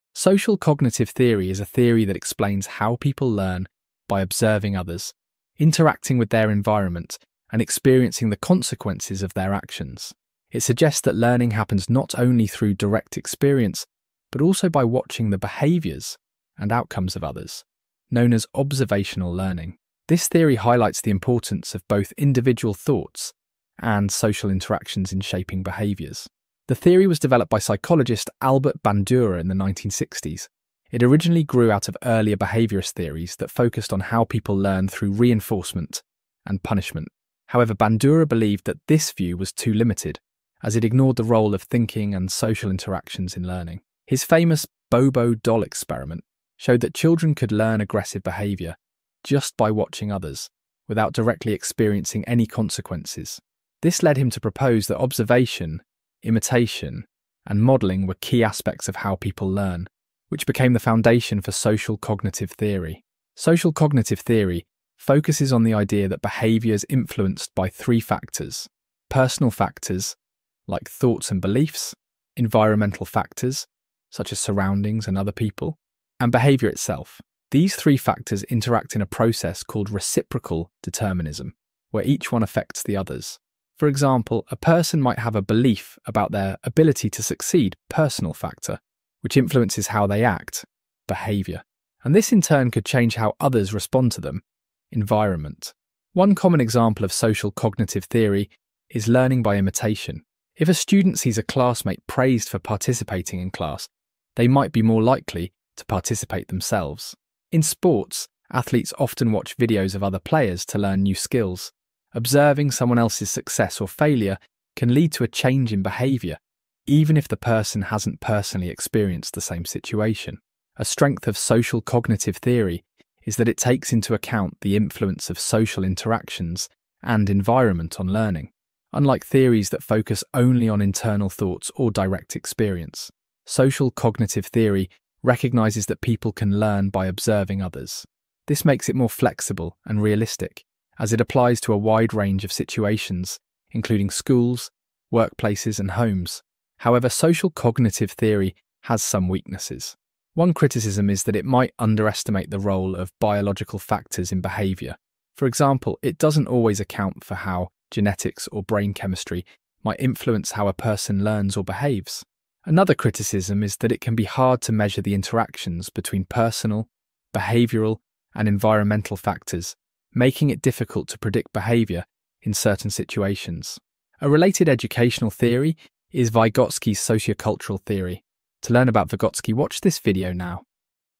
Social Cognitive Theory (Explained in 3 Minutes)
Social cognitive theory is a theory that explains how people learn by observing others, interacting with their environment, and experiencing the consequences of their actions. It suggests that learning happens not only through direct experience but also by watching the behaviors and outcomes of others, known as observational learning. This theory highlights the importance of both individual thoughts and social interactions in shaping behaviors.